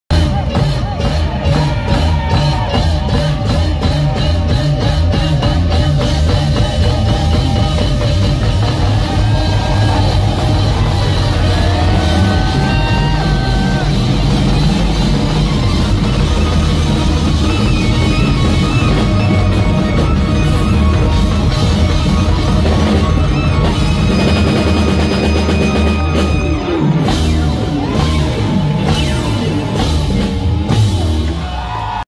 Stephen Devassy Mass Ending | Kollam | Bishop Jerome Institute | Cogniz' 19
ഞങ്ങളുടെ കോളേജിൽ ( ബിഷപ്പ് ജെറോം ഇൻസ്റ്റിറ്റ്യൂട്ട്, കൊല്ലം) 2019-ൽ നടത്തിയ Cogniz'19  ടെക് ഫെസ്റ്റിന് നടത്തിയ സ്റ്റീഫൻ ദേവസ്സി യുടെ സംഗീത പരിപാടിയുടെ ഒരു തകർപ്പൻ അവസാന രംഗമാണ് ഇത്.